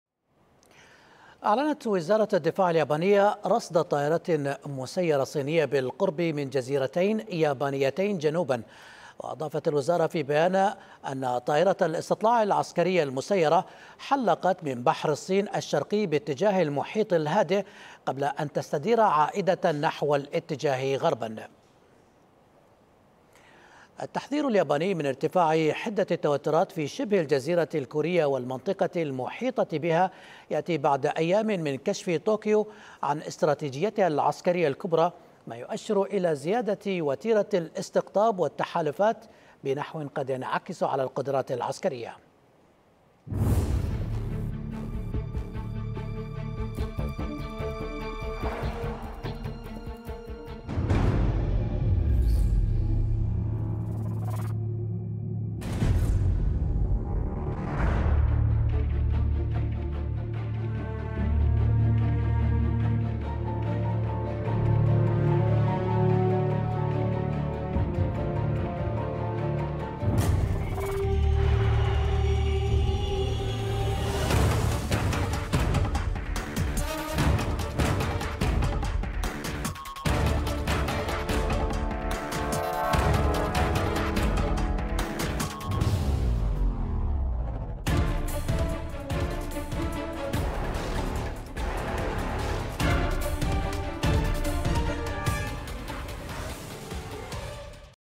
اليابان: رصد مسيرة عسكرية صينية قرب جزر يابانية جنوبية - أخبار الشرق
أعلنت وزارة الدفاع اليابانية رصد طائرات مسيرة صينية بالقرب من جزيرتين يابانيتين جنوبا، وأضافت الوزارة في بيان أن طائرة الاستطلاع العسكرية المسيرة قد حلقت من بحر الصين الشرقي باتجاه ال محيط الهادئ قبل أن تستدير عائدة نحو الاتجاه غربا.